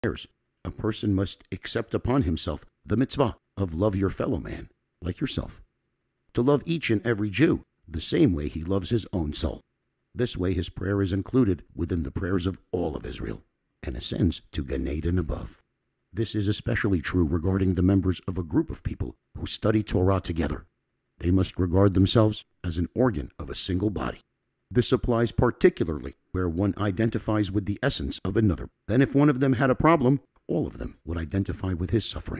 reMix- the Importance of Praying for You Fellows
Love your fellow Jew like you love yourself - when we come together as one, our prayers ascend to Gannaden above.